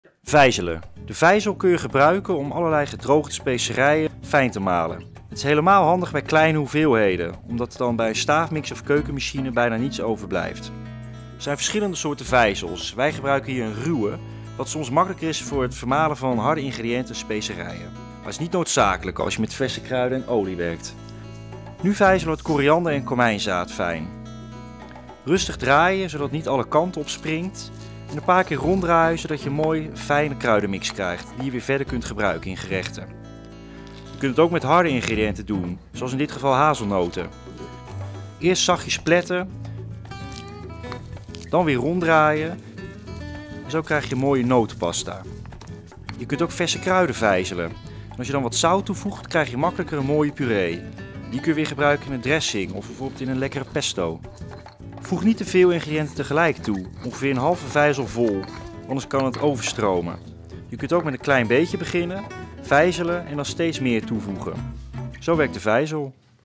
Vijzelen met een granieten vijzel - Allerhande
Door vijzelen kun je onder andere gedroogde specerijen fijnmalen. Er zijn verschillende soorten vijzels en verschillende vijzeltechnieken. Allerhande geeft in dit filmpje met een granieten vijzel enkele tips en trucs.

De vijzel kun je op verschillende manier gebruiken. We laten je eerst zien hoe je een fijne kruidenmix maakt van koriander en komijnzaad. Vervolgens zie je hoe van harde hazelnoten een mooie notenpasta gemaakt wordt. Eerst rustig pletten en vervolgens fijnmalen tot de pasta ontstaat die je in gerechten kunt gebruiken. Tot slot vijzelen we ook verse kruiden met zout tot een fijne puree. Vul de vijzel voor ongeveer de helft met de ingrediënten, zodat het niet overstroomt.

De meeste kookhulp en receptinspiratie vind je iedere dag op Allerhande Online. Bezoek ook eens